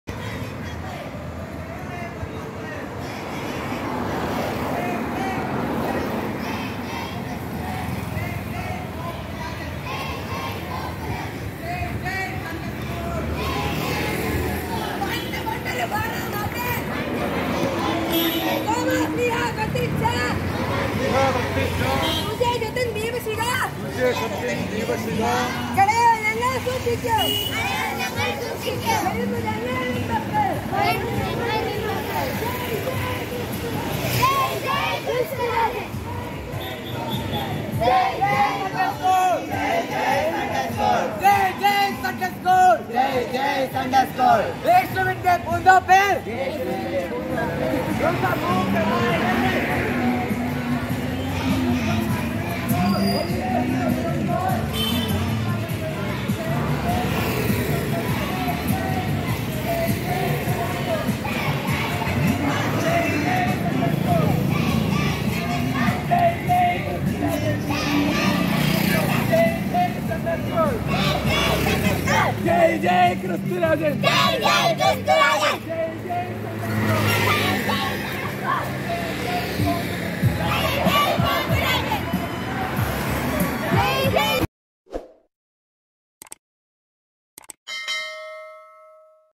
എല്ലാ കുഞ്ഞു കൂട്ടുകാർക്കും സണ്ടേസ്കൂൾ ദിനത്തിന്റെ ആശംസകൾ|Sunday School day |Fr.Godwin Christo OIC
മലങ്കര സഭയിലെ സണ്ടേസ്കൂൾ ദിനമായ ഇന്ന് മണ്ണന്തല St.John Paul II ദൈവാലയത്തിൽ നടന്ന മനോഹരമായ റാലി |St. John Paul 2 Mannanthala TVM